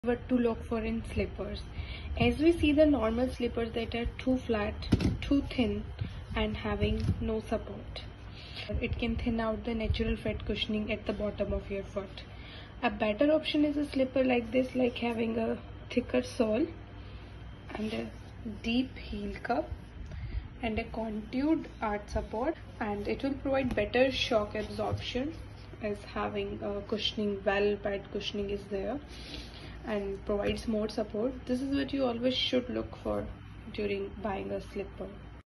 What Slippers to Buy ? - Regular v/s Shapecrunch Arch Support Slippers for Women and Men
What slippers to buy ?
Your posture begins at Feet.
Most slippers do more harm than providing comfort - causing flat feet, plantar fasciitis and other problems.
Most ergonomic and orthopedic slippers aren’t attractive. Shapecrunch has a great range that provides style with comfort.

Shapecrunch makes comfortable slippers for Men and Women. Shapecrunch is best known for its Orthopedic Slippers for Pain relief, Posture Correction and and Comfort.
Shapecrunch slippers come with
1. Arch Support
2. Heel Cup
3. Cushioning
4. Wide toe space

These are best cushioned slippers for Heel Pain and Flat Foot. They come in different colors so you can buy according to youe choice. Shapecrunch provides steady and balanced base for the perfect posture.

to check out all designs.